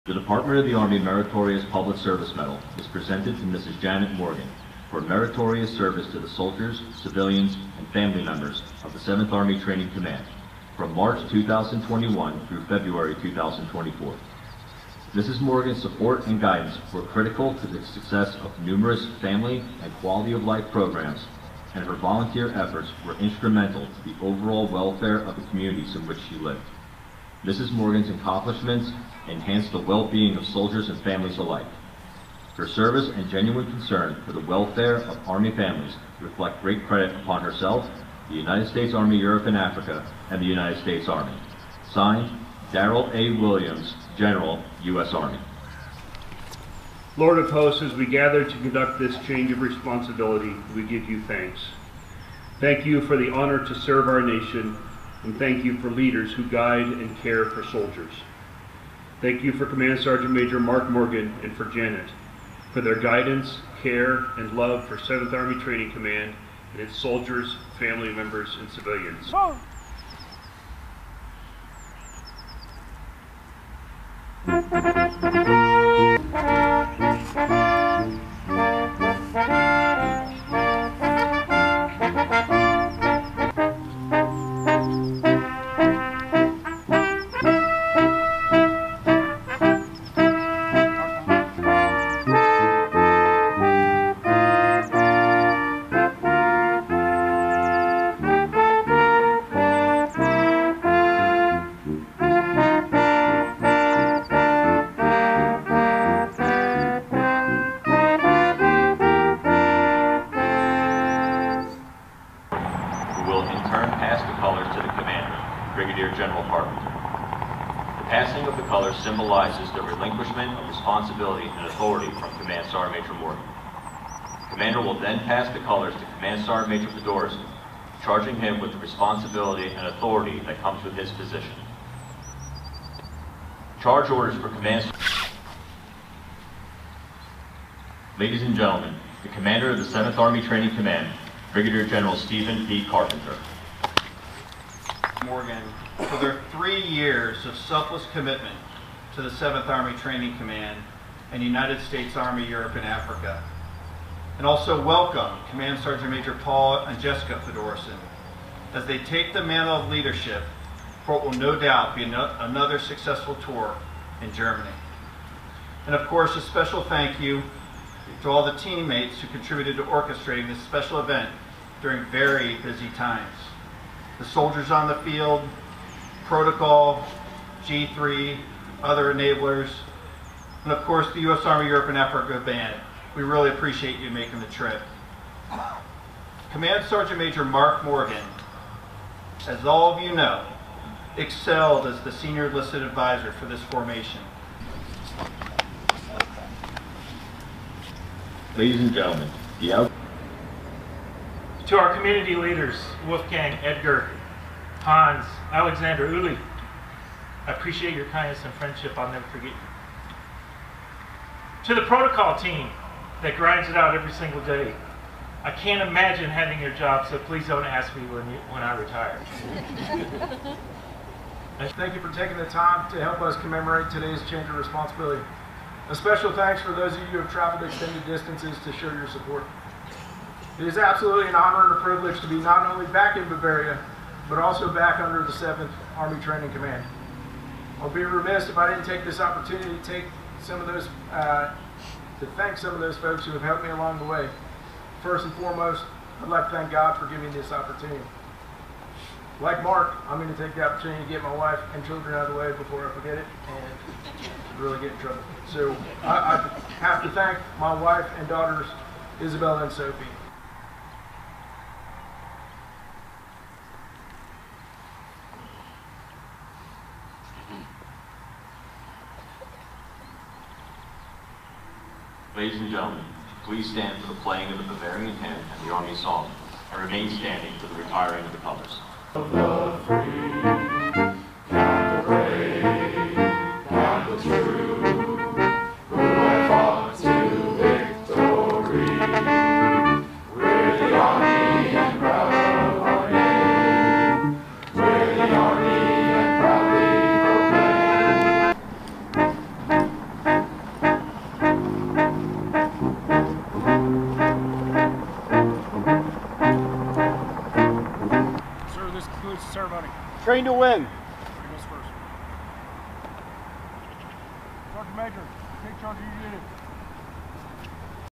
7th Army Training Command Change of Responsibility Ceremony | Grafenwoehr Parade Field
The 7th Army Training Command (7ATC), hosts a Change of Responsibility Ceremony at the Grafenwoehr Parade Field in Grafenwoehr, Germany, Feb. 15, 2024.
7th Army Training Command Change of Responsibility Ceremony | Grafenwoehr Parade Field | Feb. 15, 2024

7ATC Change of Responsibility Ceremony
BY, GERMANY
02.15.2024

Outgoing Command Sgt. Maj. Mark A. Morgan relinquished his responsibility to incoming Command Sgt. Maj. Paul M. Fedorisin. 7ATC enables readiness for all assigned and regionally aligned United States Army Europe and Africa forces, Allies, and Partners by conducting live, virtually, and constructing training in a fully replicated, multi-domain battlefield. (U.S. Army video by Spc. Adrian Greenwood)

Film Credits: Video by Spc. Adrian Greenwood
7th Army Training Command